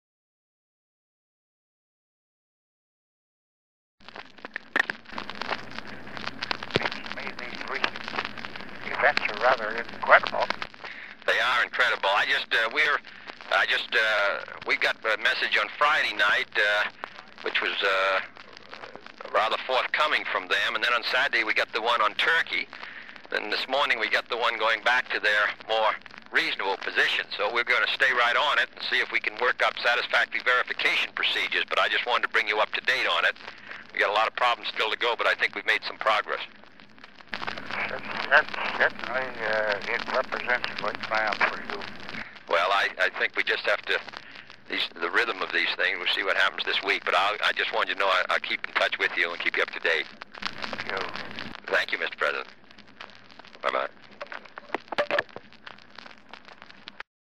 KENNEDY TAPES: Herbert Hoover on Cuban Missile Crisis
JFK & Herbert Hoover
Dictabelt L41.4
October 28, 1962
White House Telephone

President John F. Kennedy and former president Herbert Hoover discuss the Cuban Missile Crisis.

(It is difficult to hear President Hoover in this conversation. I did, however, modify the recordings slightly to raise the volume when Hoover speaks.)

(Photo: Presidents Herbert Hoover & John Kennedy.)